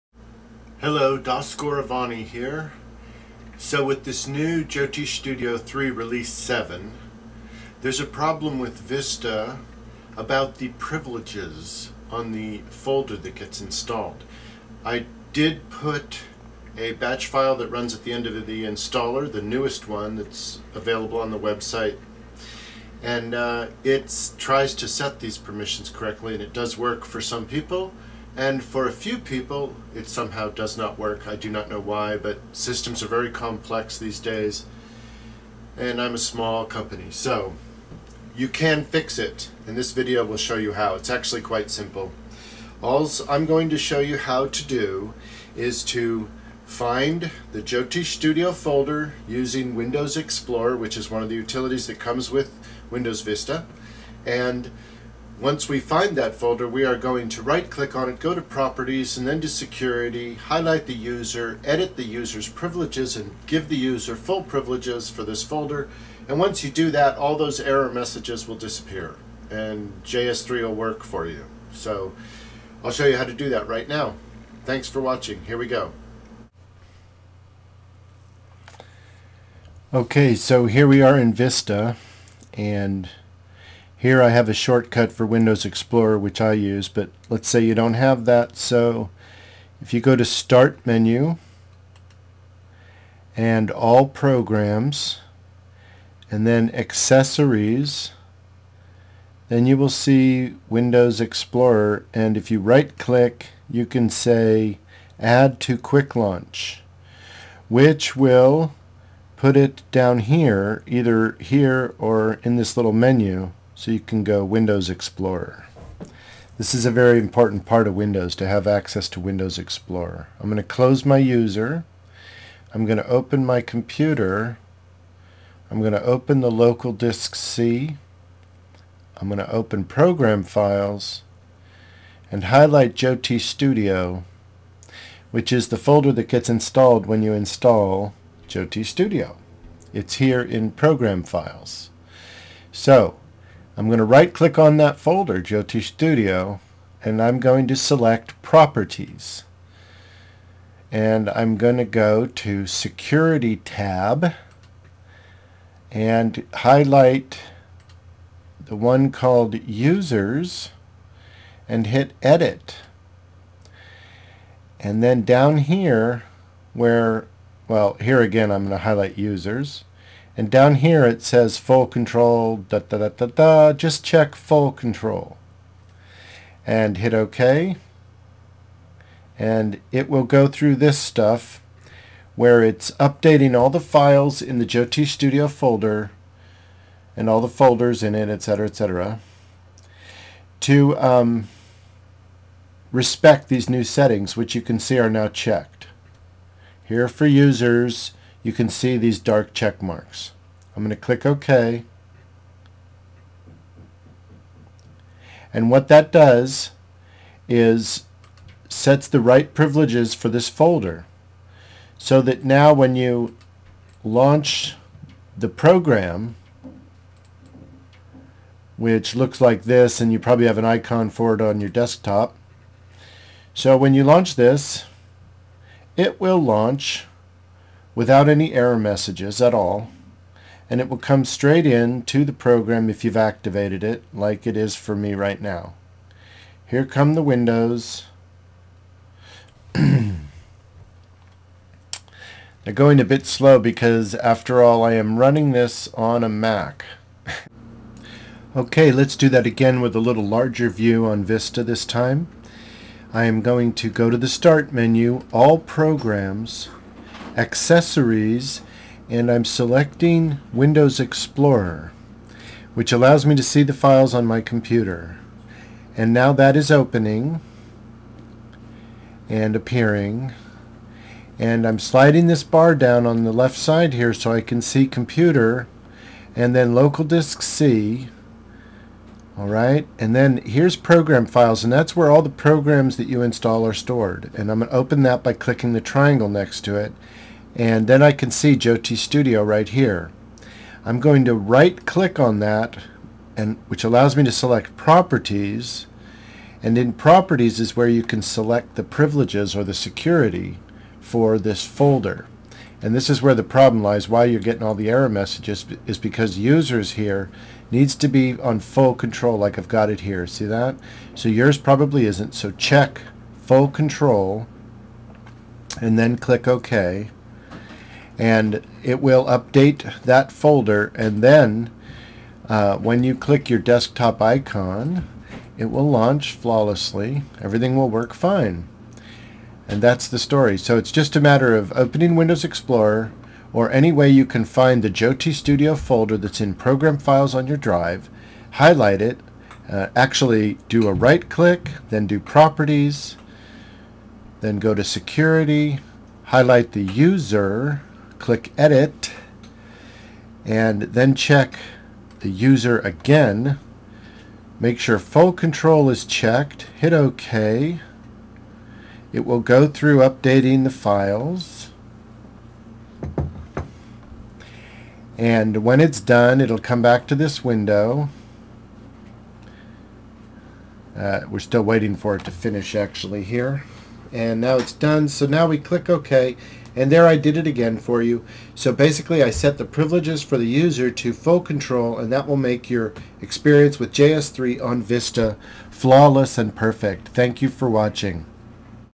Vedic Astrology Software - Jyotish Studio - Vista Errors Fix for JS 3 R7
Shows you how to manually and easily fix the CURRENT PROBLEM with VISTA where it gives lots of errors when you try to run JS3R7.  It's about privileges, or security.  You set the User in Security in Properties for the installed Jyotish Studio folder which is in Program Files.  This videos shows doing these things. By and with Das Goravani, programmer.

Das Goravani has been recognized by Shri KN Rao's Institute of Astrology in Delhi, India, for contributions to the spread of Vedic Astrology/Jyotish.